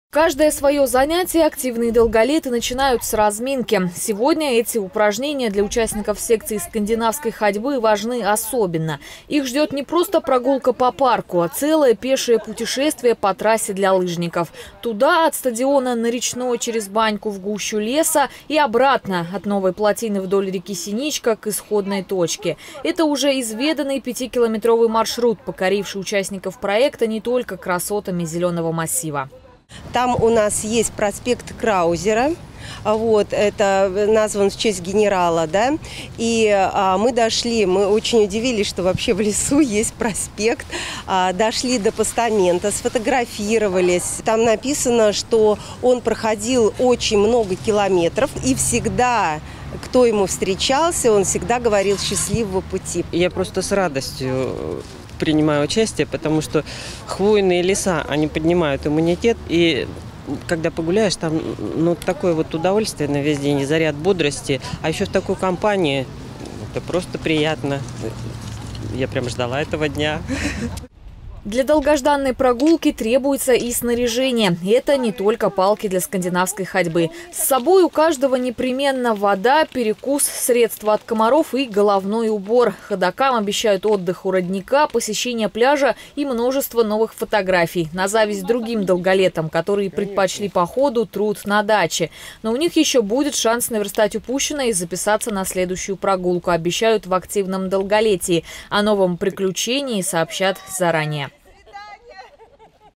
КРТВ. По лыжной трассе – только с палками
Участники проекта «Активное долголетие» прошли 5-километровый маршрут через красногорский лес